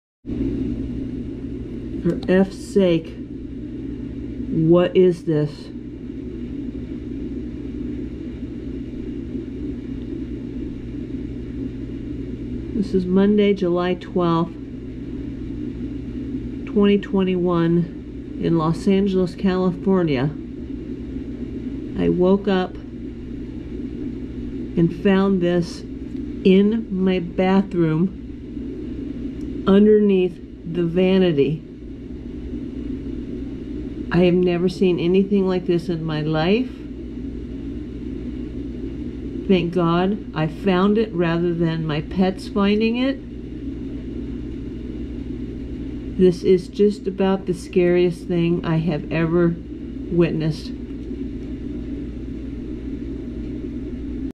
Woman Wakes Up To Find Giant Ink Cap Mushroom in Bathroom
This giant mushroom was found in a Los Angeles bathroom by a shocked woman. The floors were just cleaned days ago. This was not present the night before. This sprouted in hours! What is going on behind the walls?

Drop us a note:

April, Alfie & Ableu
11333 Moorpark Street
PMB 195
U.S.A.